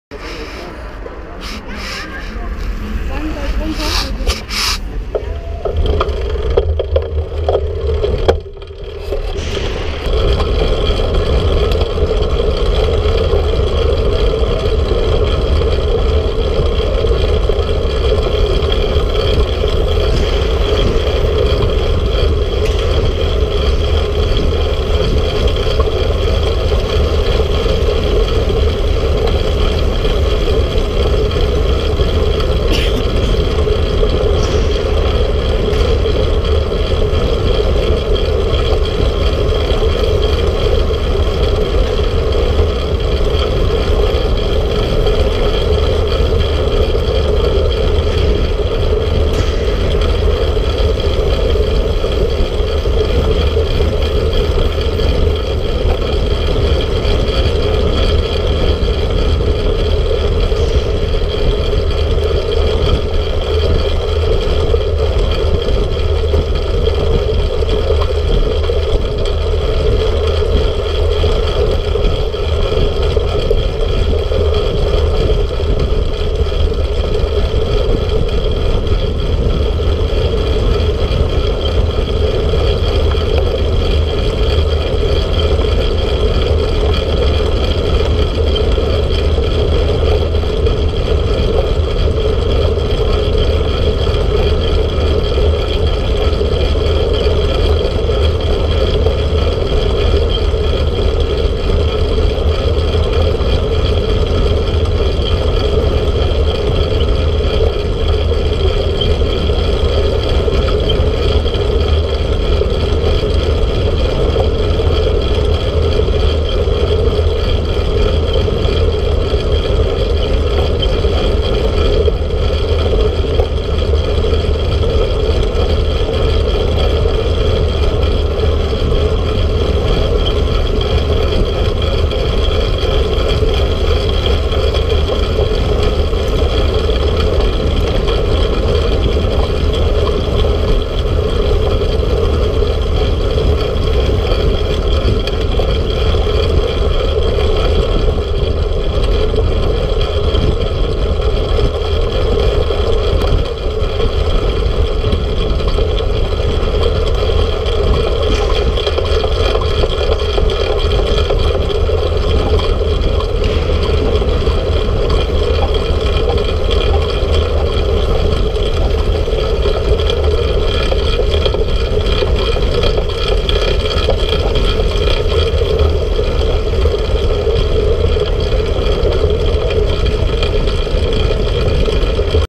Bahntraining der VfR Radsportjugend im Sportforum Kaarst - Büttgen
Warmfahren zum Start